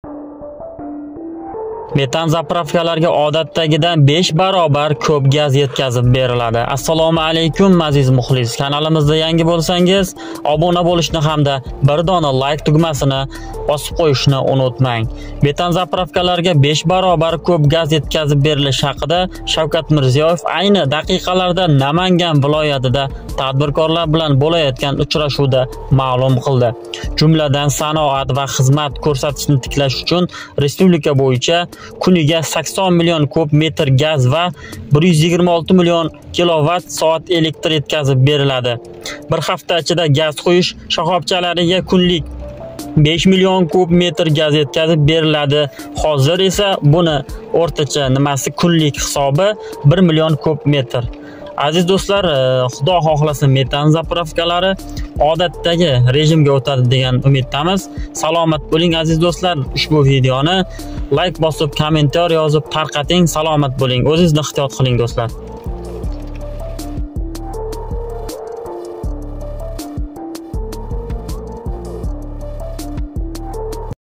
Хуш_хабар❗️Метан «заправка»ларга 5 баробар кўп газ етказиб берилади, METAN ZAPRAFKALAR ISHLADI
❗️Метан «заправка»ларга 5 баробар кўп газ етказиб берилади

Бу ҳақда Шавкат Мирзиёев айни дақиқаларда Наманган вилоятида тадбиркорлар билан бўлаётган учрашувда маълум қилди.

Жумладан, саноат ва хизмат кўрсатишни тиклаш учун, республика бўйича:

✅ кунига 80 миллион куб метр газ ва 126 миллион кило-ватт соат электр етказиб берилади;

✅ бир ҳафта ичида газ қуйиш шахобчаларига кунлик 5 миллион куб метр газ етказиб берилади (ҳозир – 1 миллион куб метр).